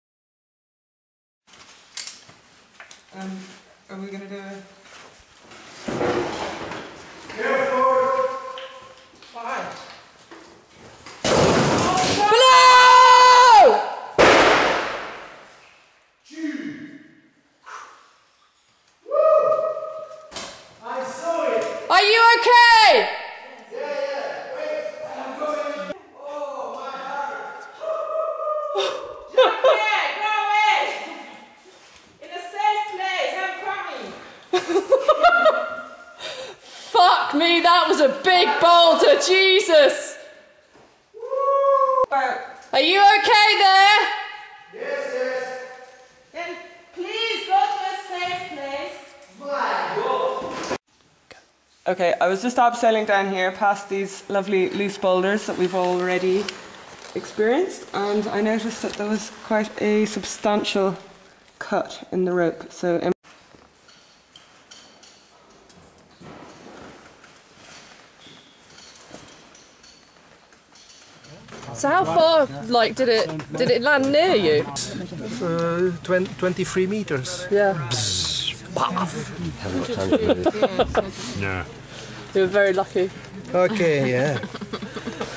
Dramatic moment a rockfalls down a pitch in remote indian cave
A rockfall from the pitch head in Meghalaya.